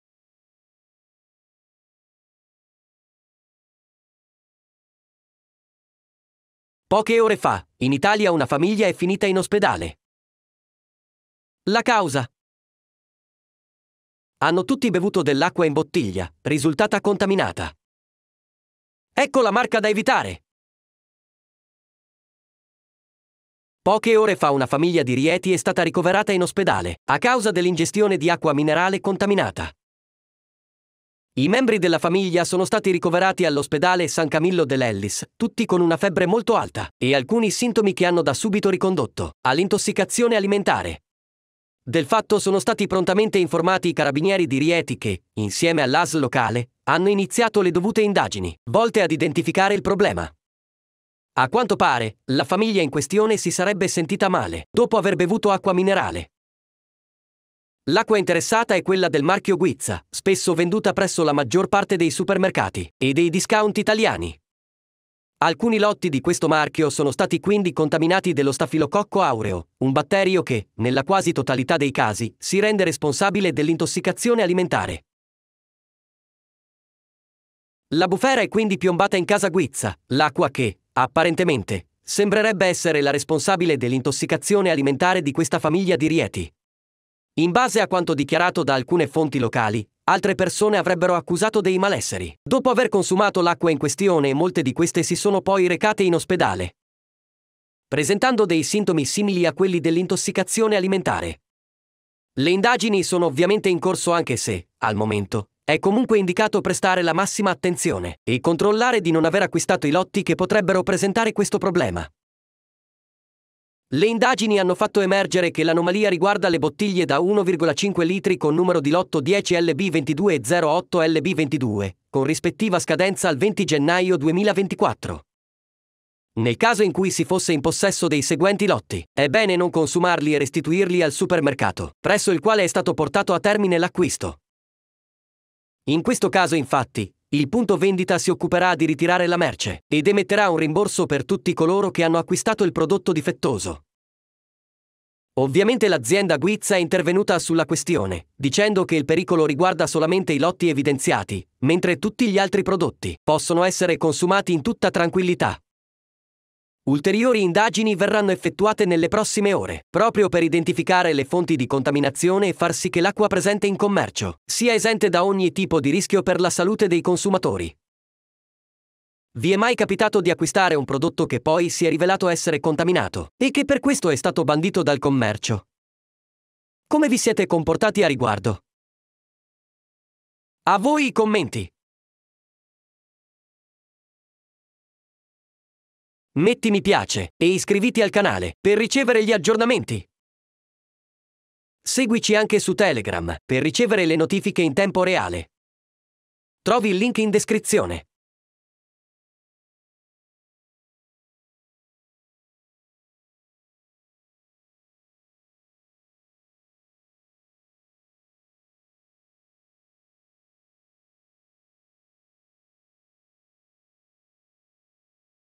Italia, Acqua Minerale: Famiglia Finisce All'Ospedale!
Poche ore fa, in Italia una famiglia è finita in ospedale. La causa? Hanno tutti bevuto dell'acqua in bottiglia, risultata contaminata. Ecco la marca da evitare!